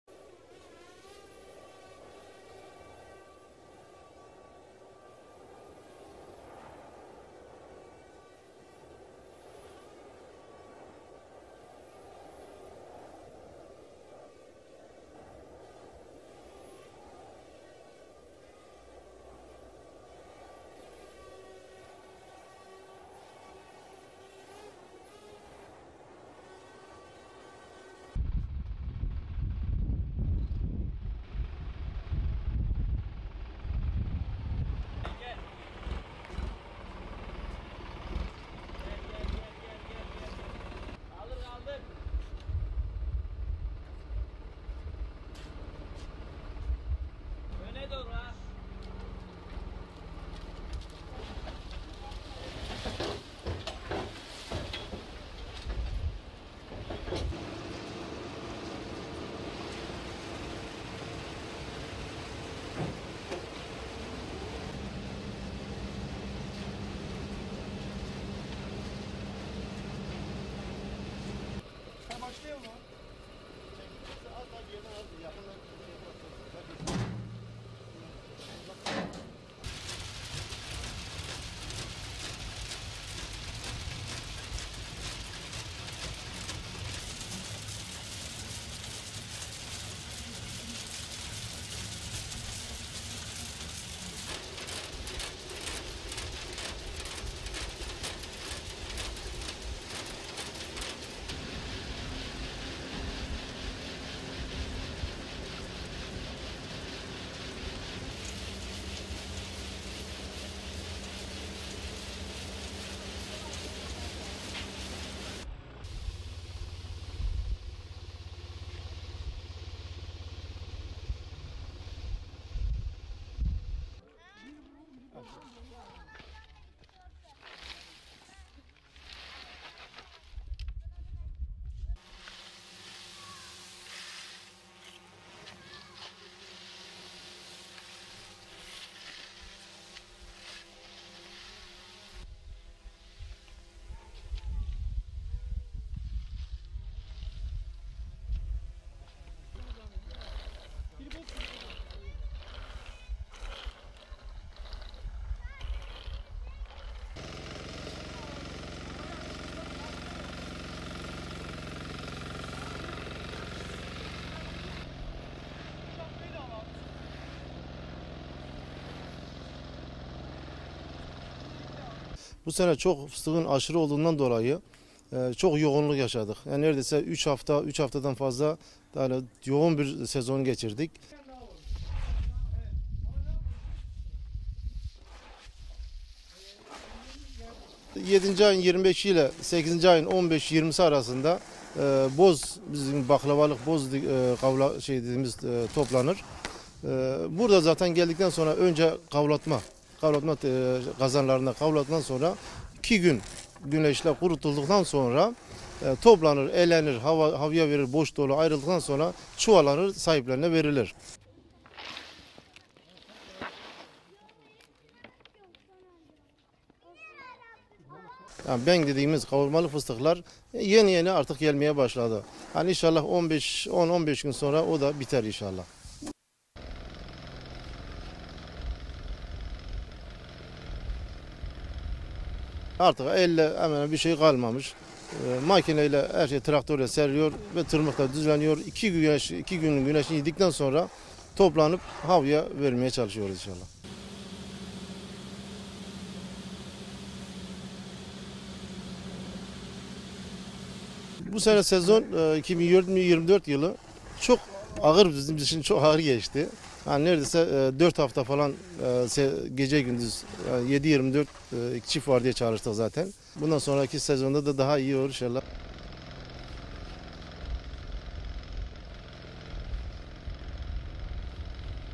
Antep Fıstığında "Kavlatma" Telaşı Sürüyor
Güneydoğu Anadolu Bölgesi'nin önemli tarımsal ürünlerinden Antep fıstığı, hasadının ardından kavlatma tesislerinde dış kabuğundan ayrılıyor